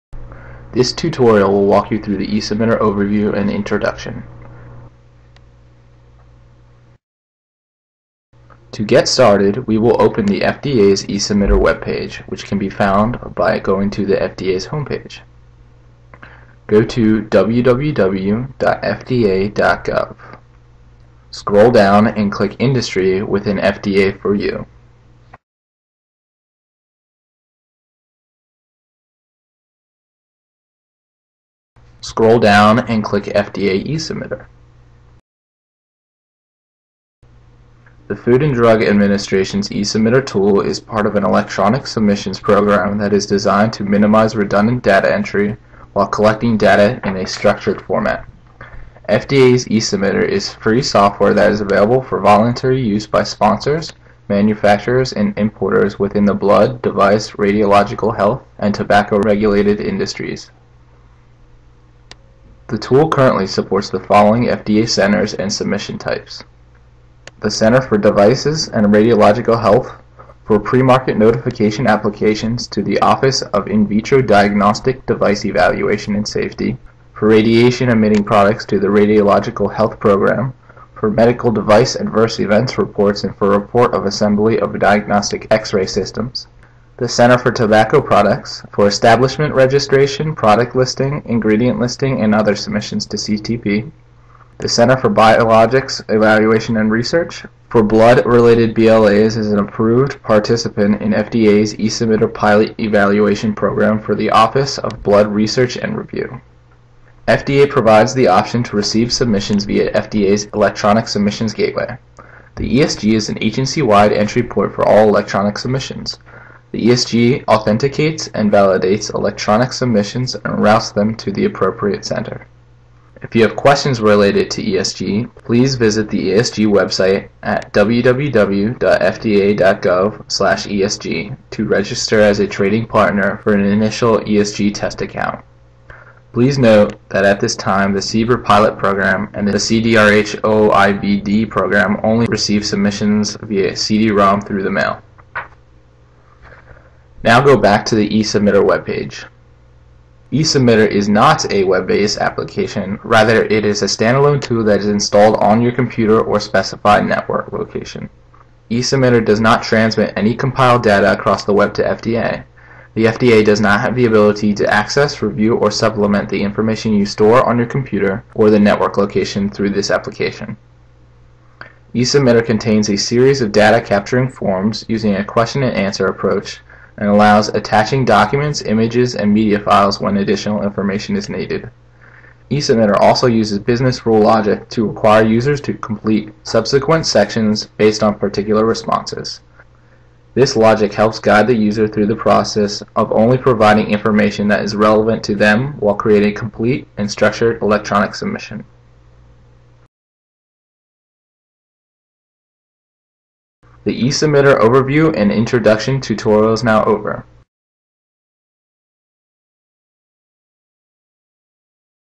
Tutorial eSubmitter Overview and Introduction.
FDA Presentation: FDA/CDRH Presentation concerning Tutorial eSubmitter Overview and Introduction.

The eSubmitter tool is designed to facilitate the electronic submission process for medical device submissions, allowing manufacturers to efficiently navigate the regulatory landscape of the FDA. This presentation aims to provide an overview of the functionalities and benefits of using the eSubmitter application.

The tutorial provides an overview of the FDA's eSubmitter tool, which is part of an electronic submissions program aimed at reducing redundant data entry and organizing data in a structured format. This free software is available for voluntary use by sponsors, manufacturers, and importers in the blood, device, radiological health, and tobacco industries.

Key Features:

Supported Centers: The eSubmitter supports various FDA centers, including:

Center for Devices and Radiological Health (pre-market notifications, adverse event reports)
Center for Tobacco Products (establishment registration, product listing)
Center for Biologics Evaluation and Research (blood-related submissions)
Electronic Submissions Gateway (ESG): The FDA offers the option to submit documents via the ESG, which authenticates and routes submissions to the appropriate center.

Standalone Application: eSubmitter is not web-based; it is installed locally and does not transmit data over the internet. The FDA cannot access or review the information stored on users' computers.

Data Capture Forms: The tool includes a series of forms that use a question-and-answer format, allowing users to attach documents and media. It employs business rule logic to guide users based on their responses, ensuring that only relevant information is provided for a complete submission.

The tutorial concludes with a reminder that eSubmitter is designed to streamline the submission process while maintaining data security and relevance.

Objective of eSubmitter

The primary goal of eSubmitter is to support industry stakeholders by streamlining the submission process. By leveraging this tool, users can ensure that their submissions adhere to FDA guidelines, thereby reducing administrative burdens and minimizing the potential for errors.

Key Features of eSubmitter

1. User-Friendly Interface: The eSubmitter interface is intuitive, allowing users to easily input data and upload necessary documentation. This design is intended to enhance user experience and minimize learning curves for new users.

2. Guidance Through the Submission Process: The tool provides step-by-step guidance to assist users in completing their submissions accurately. This includes prompts for required information and helpful tips that streamline the data entry process.

3. Validation Checks: eSubmitter incorporates built-in validation checks that evaluate the completeness and accuracy of submissions before they are sent to the FDA. This proactive approach helps prevent rejection due to common errors.

4. Real-Time Submission Tracking: Users can monitor the status of their submissions in real-time, enabling better communication and transparency with the FDA throughout the review process.

5. Resource Access: eSubmitter connects users to a wealth of resources, including instructional materials, webinars, and FAQs that can enhance comprehension of FDA requirements.

The eSubmitter tool represents a significant advancement in the modernization of the submission process for medical devices. By incorporating features that focus on user-friendliness, real-time guidance, and efficient validation, it plays a critical role in enhancing the overall submission experience. As the landscape of regulatory submissions continues to evolve, leveraging tools like eSubmitter will be essential for manufacturers seeking to navigate their responsibilities effectively. Thank you for your attention, and we encourage all stakeholders to engage with this powerful tool to optimize their submission processes.